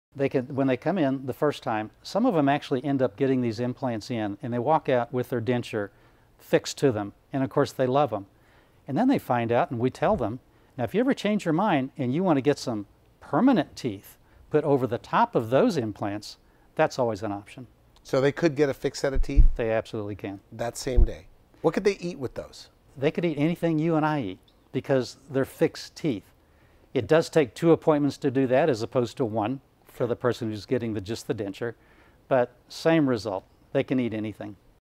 Options For Denture Wearers, James A. Fregia DDS, West Texas Dentist
Randy Alvarez interviews West Texas Dentist James A. Fregia DDS, Discussing Options for Denture Wearers /